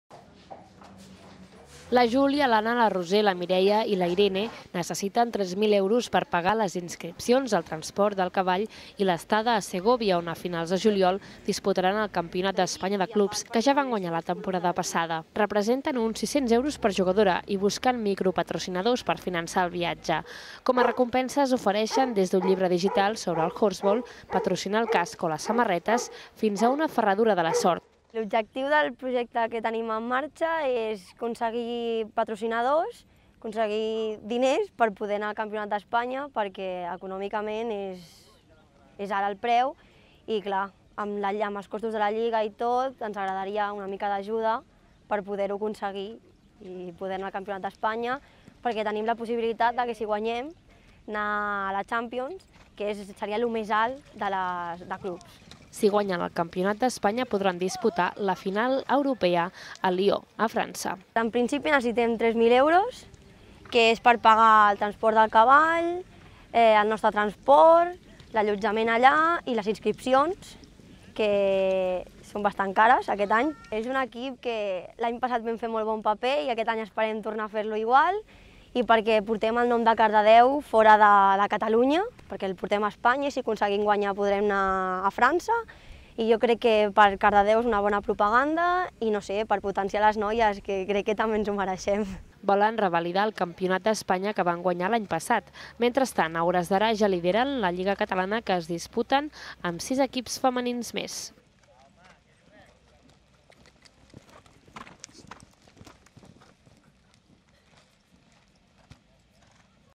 L'equip femení de horseball fa una crida al micromecenatge per anar al campionat d'Espanya
L'equip femení de horseball de Cardedeu busca mecenes per poder anar als campionats d'Espanya, del qual són vigents campiones. Han encetat una campanya de micromecenatge per finançar-se el viatge i busquen el suport dels veïns de Cardedeu peruqè asseguren que portaran el nom de la vila fins a la màxima competició europea.